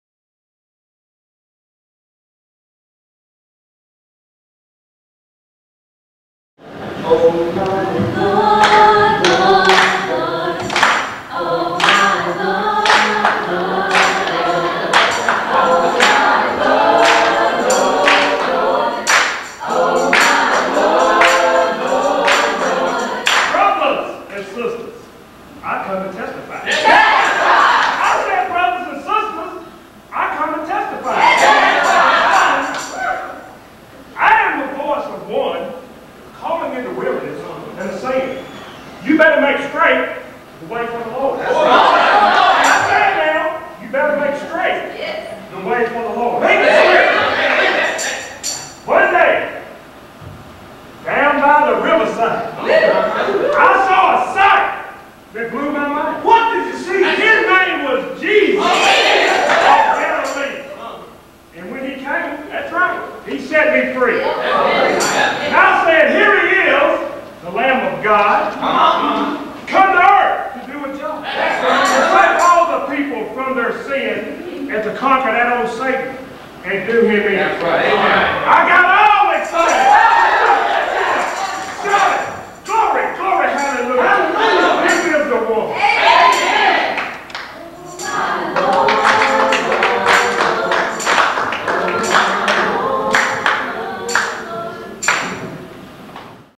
Testify
"A Portrait of Christ " by Todd Starnes; performed by Act 2 Dramatic Arts Ministry; John the Baptizer/Evangelist: Jackie Briggs